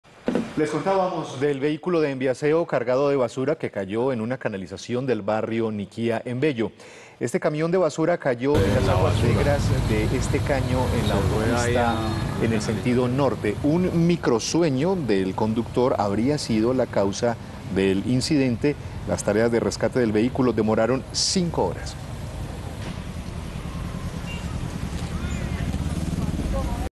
5 horas de rescate de camión de basura en canalización de Niquí
Un vehículo de Emviaseo, cargado de basura, cayó en una canalización del barrio Niquía, de Bello. El camión de basura cayó en las aguas negras de este caño en la autopista, sentido norte. Un microsueño del conductor sería la causa del incidente. Las tareas de rescate del vehículo demoraron 5 horas.